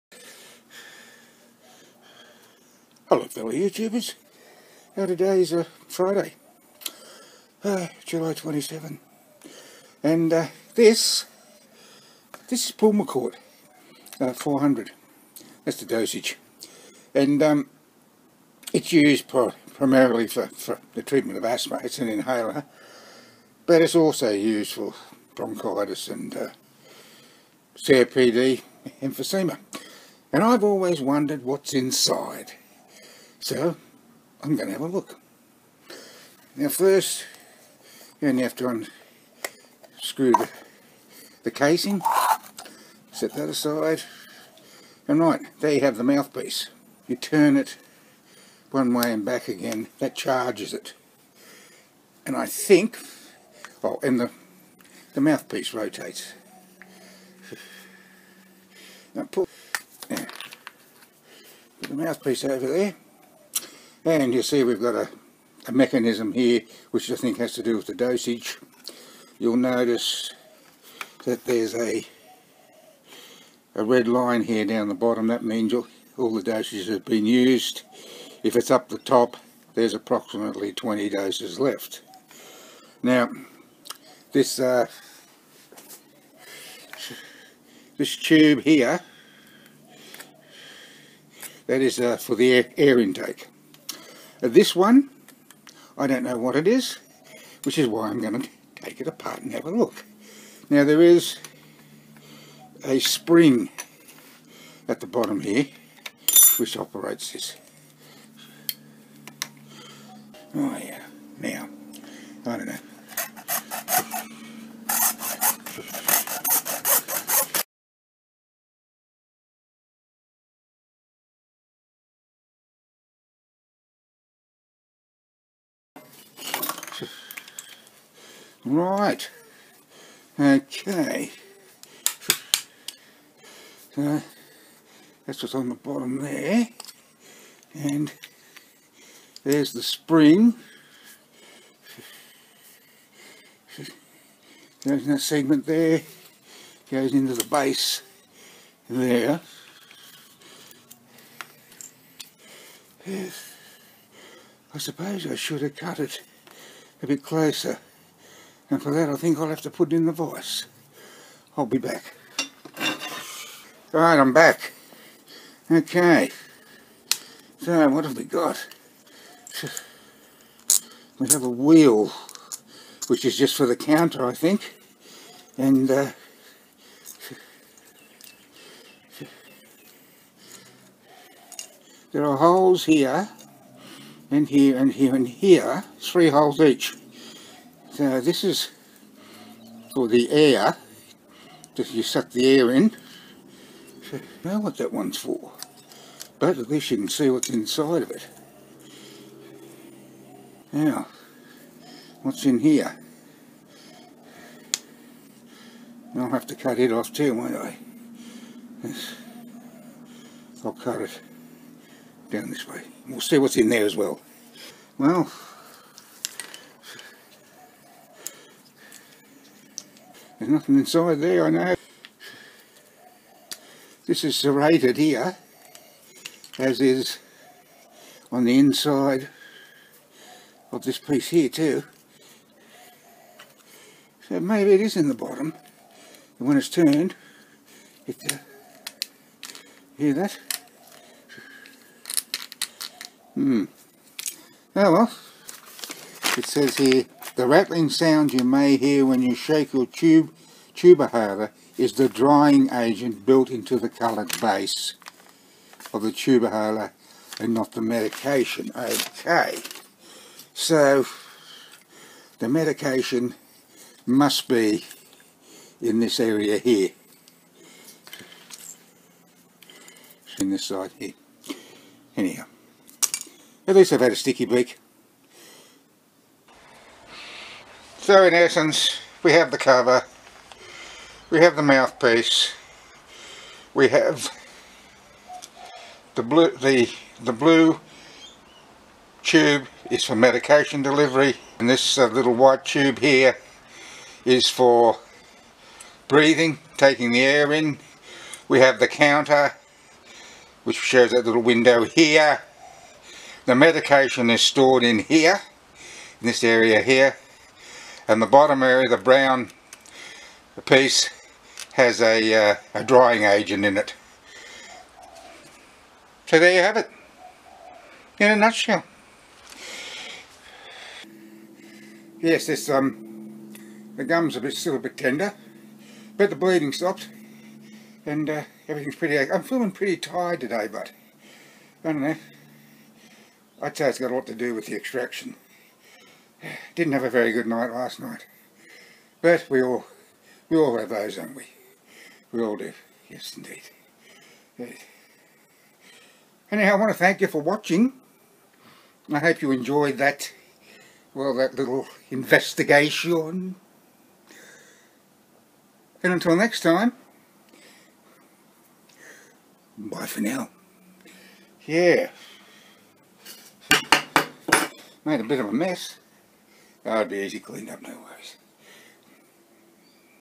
What's Inside Pulmicort Turbuhaler? (Powdered Inhalation Medication) 🙄
I've been wondering for quite some time what's on the inside of a Pulmicort Turbuhaler; so today, Friday, July 27, I decided to find out. I hope you enjoy. 🙂👍